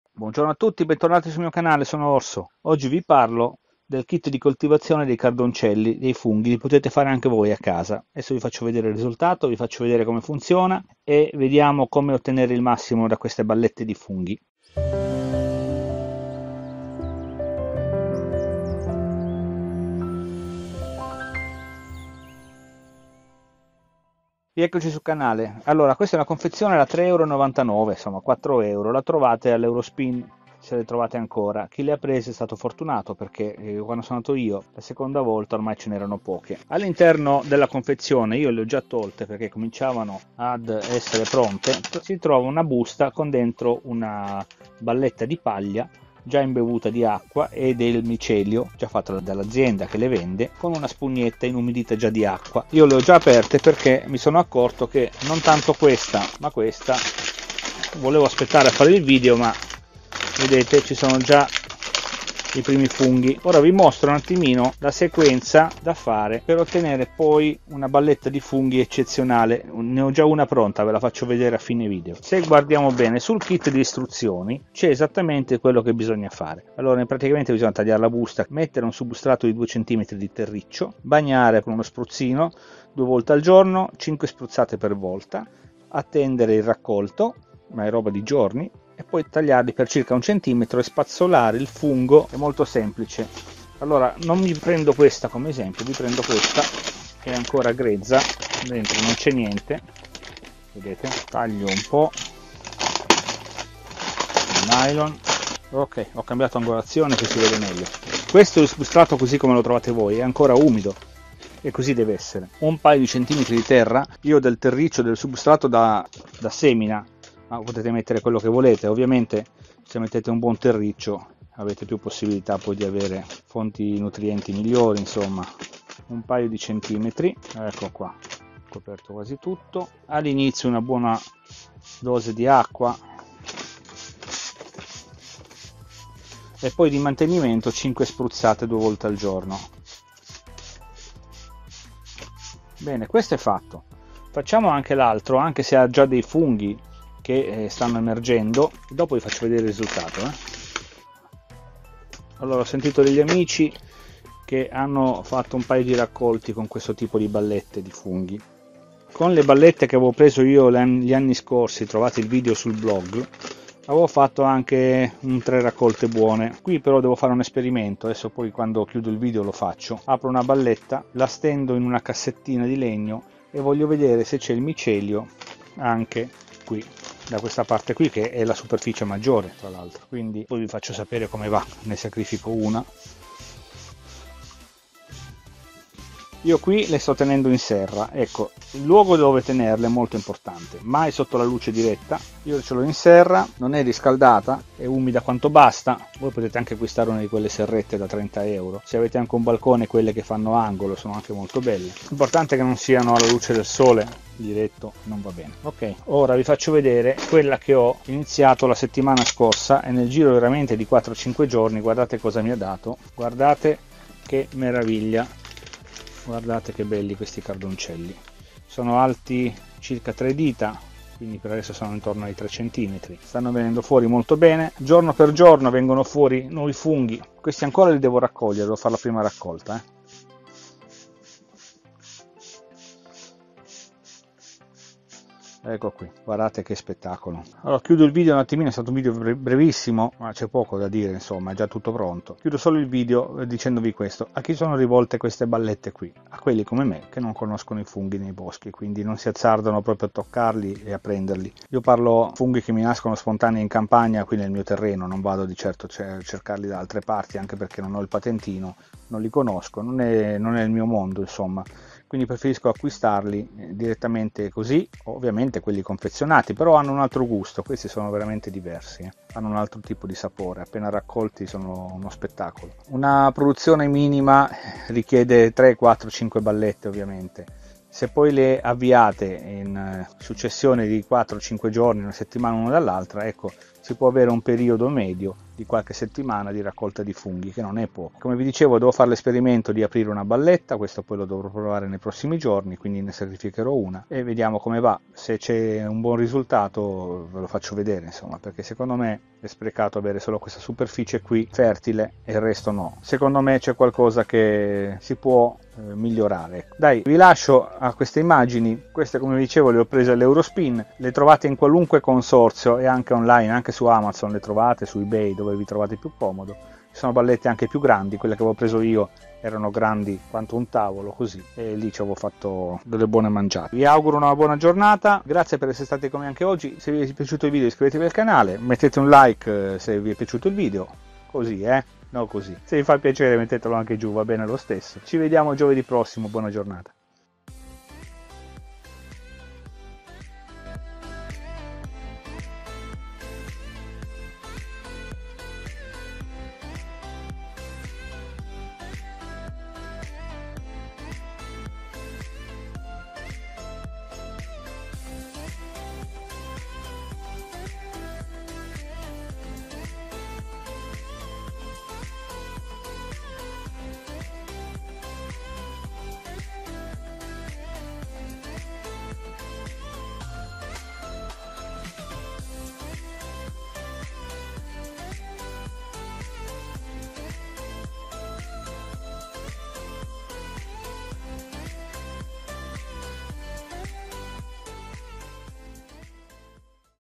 Come coltivare I FUNGHI CARDONCELLI in casa con le ballette già pronte!
Coltivare in casa propria i funghi è un'operazione semplice ed alla portata di tutti! Vi mostro la coltivazione dei funghi cardoncelli.